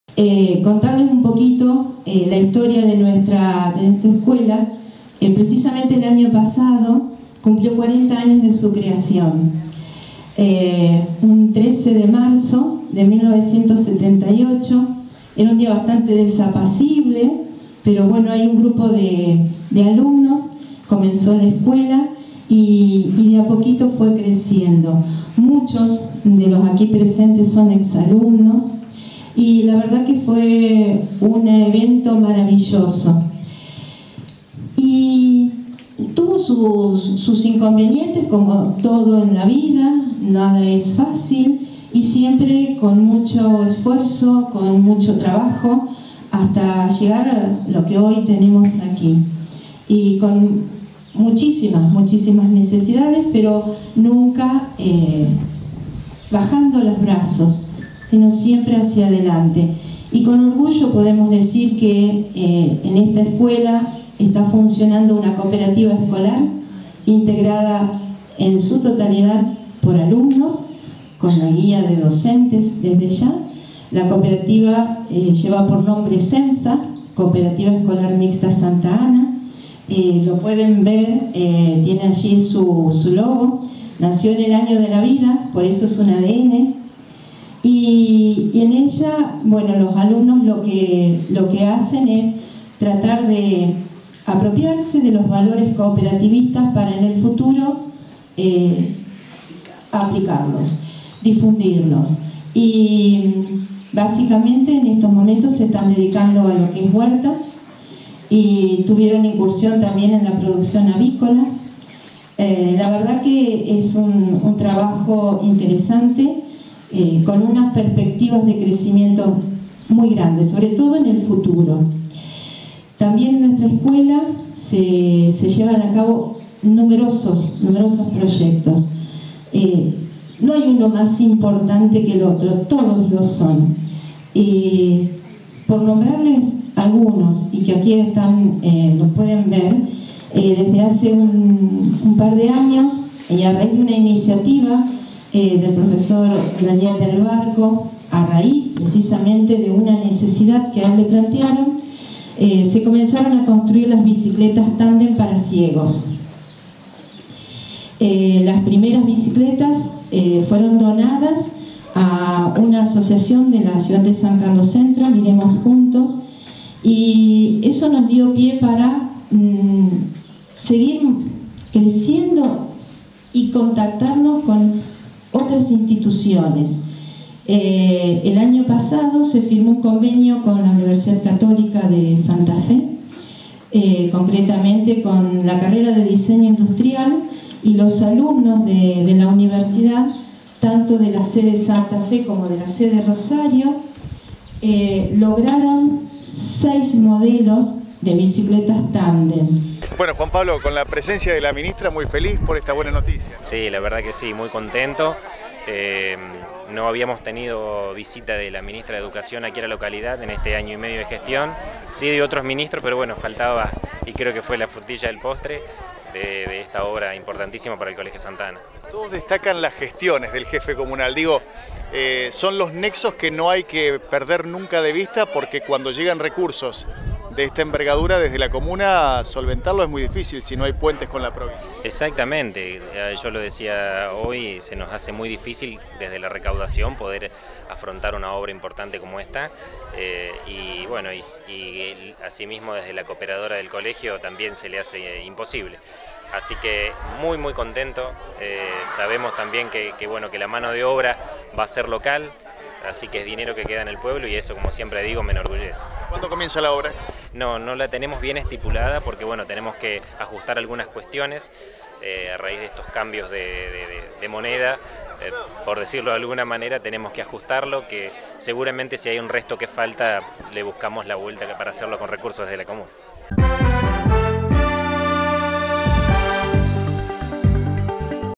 VIVIANA IMHOFF – DIRECTORA – JUAN PABLO ALARCÓN – PTE. COMUNAL
RECIBIERON UN SUBSIDIO DE $400.000 PARA EL SUM DE LA ESCUELA SANTA ANA